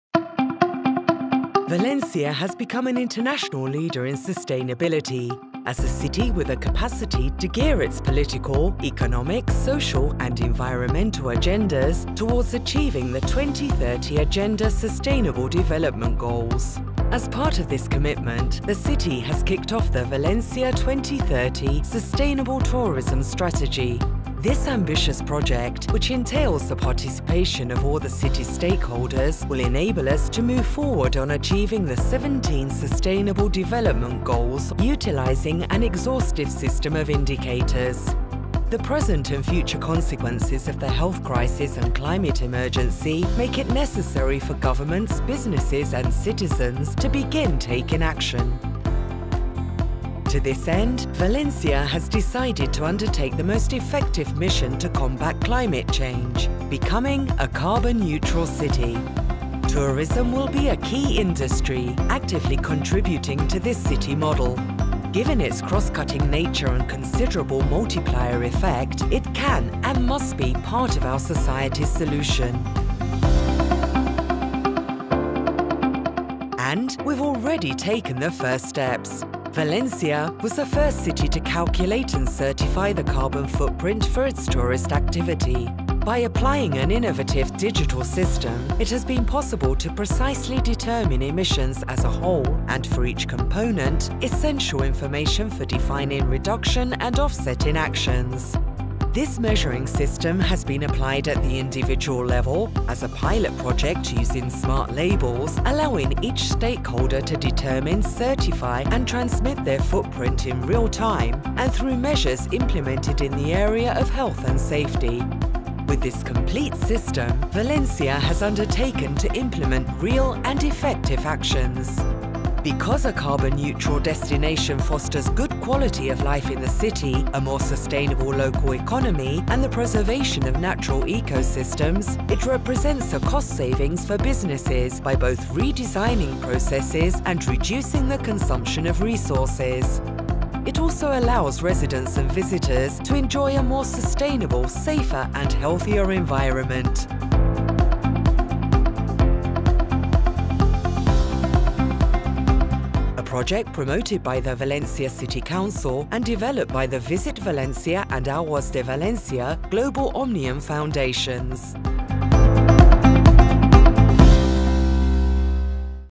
València, sustainable, safe and healthy city
In this video we present the València 2030 sustainable tourism strategy, an ambitious project, based on the participation of all the city's stakeholders, which will allow us to advance towards the fulfilment of the 17 Sustainable Development Goals (SDGs).

The present and future consequences of the health crisis and the climate emergency make it mandatory for governments, businesses and citizens to take action.  València has therefore decided to undertake the most effective mission to combat climate change: to become a carbon neutral city.

A project promoted by València City Council, and developed by the Visit València and Aguas de Valencia/ @Global Omnium Foundations.